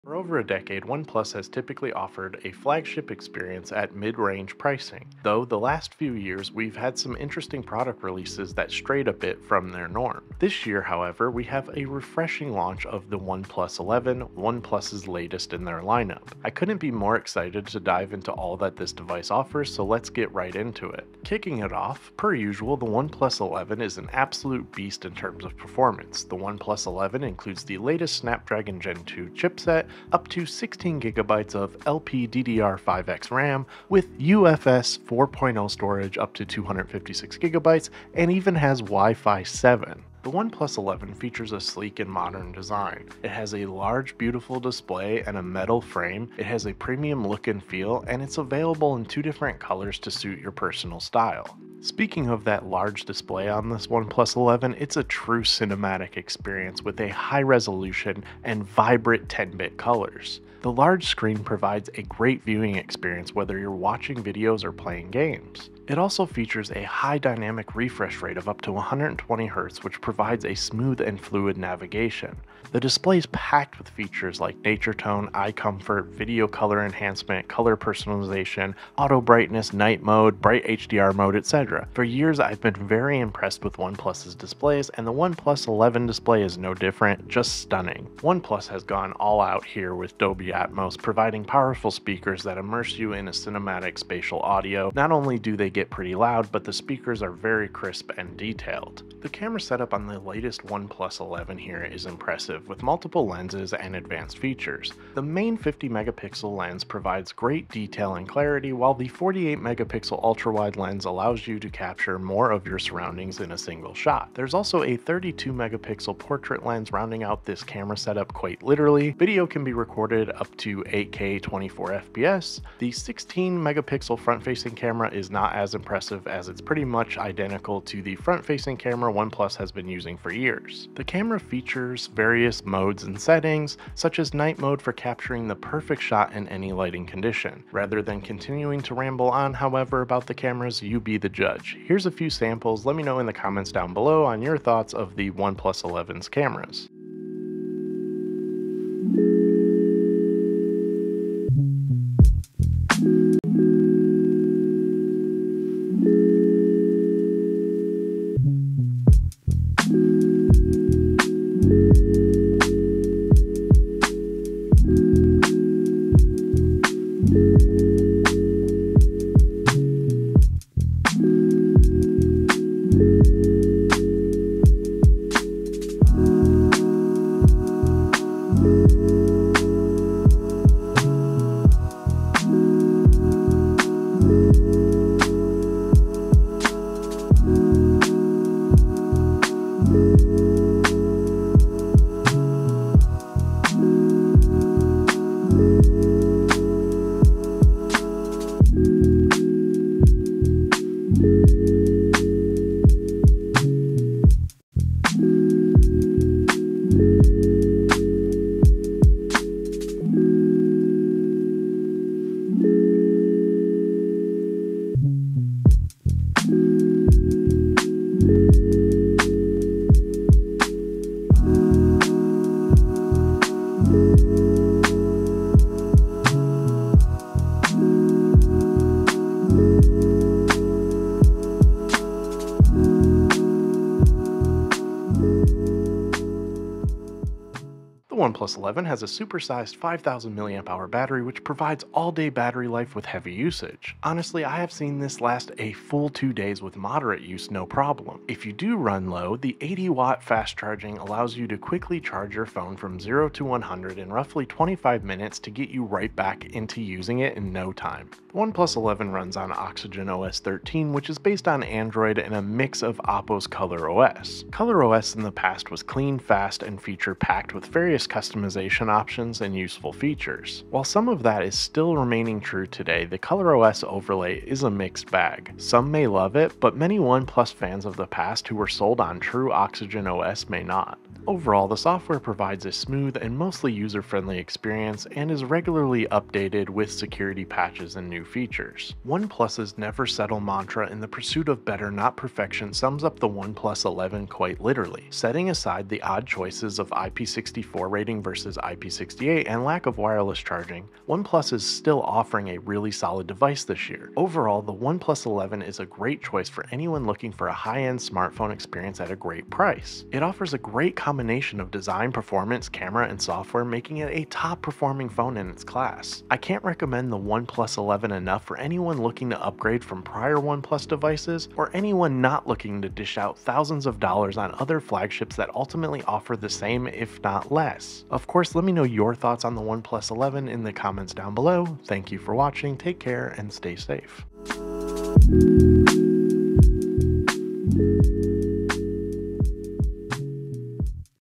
OnePlus 11 - Outstanding Return to Excellence
OnePlus has quickly shifted back to their roots with the OnePlus 11.  It's an outstanding device delivering back on OnePlus's promise to never settle!